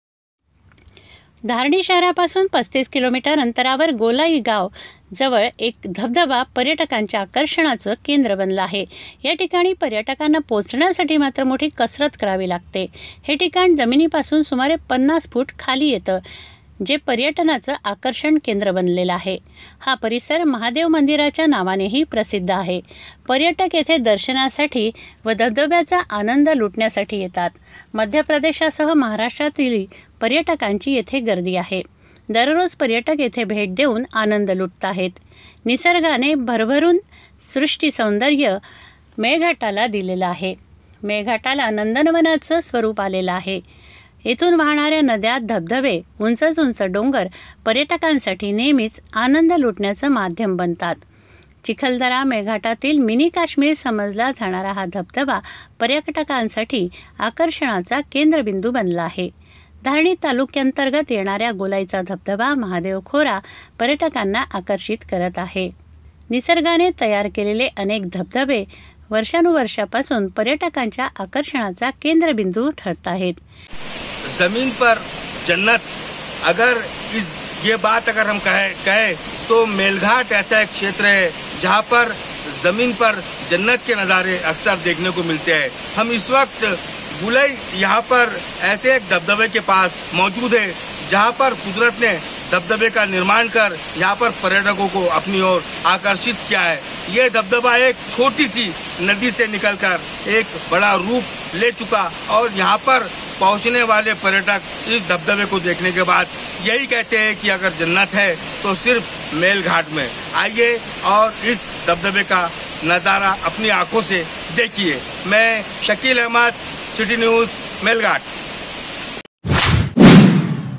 धारणीचा गोलाईचा धबधबा आकर्षणाचे केंद्र
धबधबा परिसर महादेवाच्या नावाने प्रसिद्ध
धारणीपासून पस्तीस किमी अंतरावर गोलाई
मेळघाटातील सृष्टी सौंदर्य म्हणजे प्रति स्वर्ग
9 FOR BULETIN WHATSAPP MASSEGE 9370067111#citynews_amravati